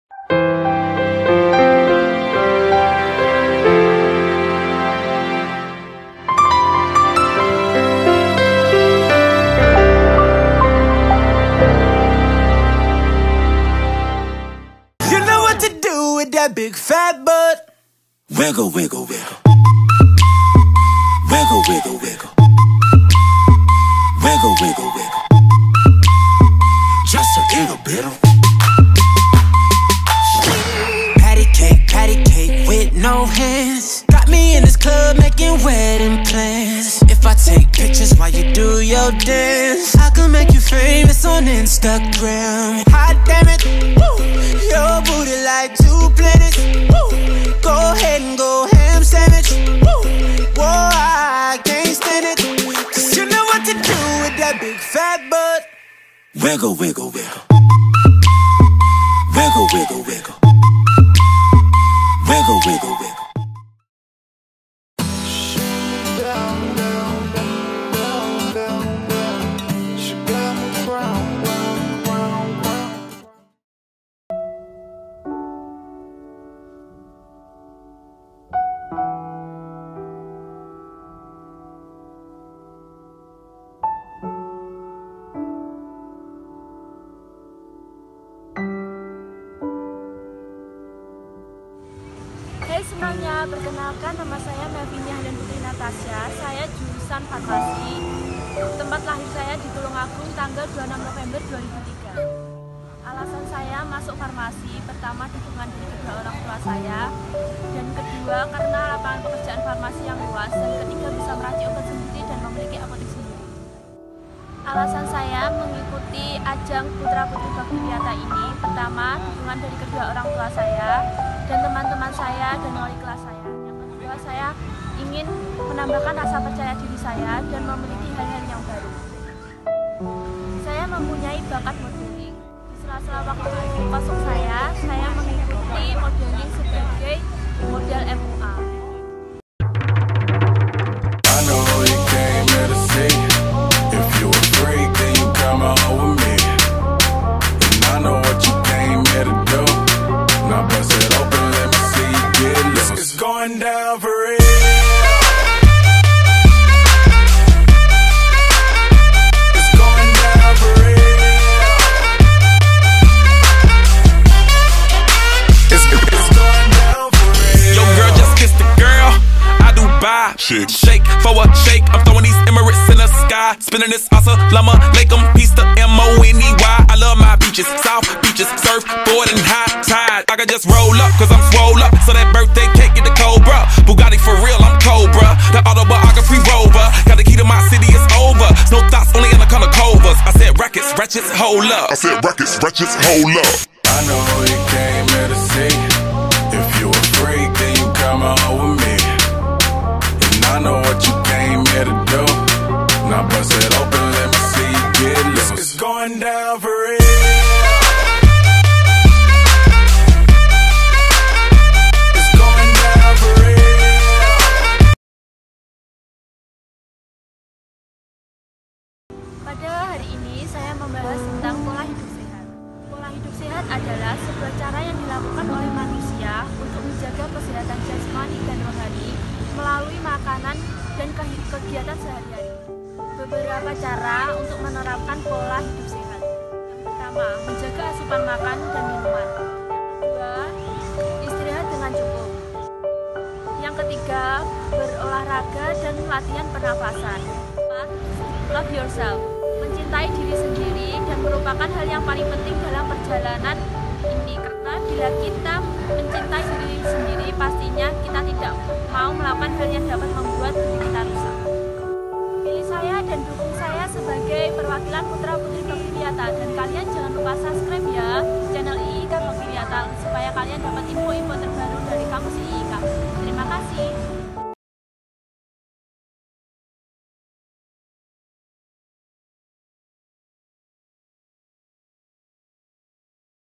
Finalis PUTRI SMKK BW 2020-2021 | Melvinia Helen Putri Natasya - Progli Farmasi
Halo warga IIK Bhakti Wiyata dan SMK Kesehatan Bhakti Wiyata.
Yuk tunjukkan partisipasimu dengan vote finalis Mr Miss IIK dan Putra Putri Bhakti Wiyata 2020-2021.
Caranya gampang :
Vote Melvinia Helen Putri Natasya dengan berikan like di video youtube ini. Share juga video ini ke teman-teman kamu dan kumpulkan like sebanyak mungkin.
Ayo dukung temanmu jadi Juara Favorit dalam ajang ini.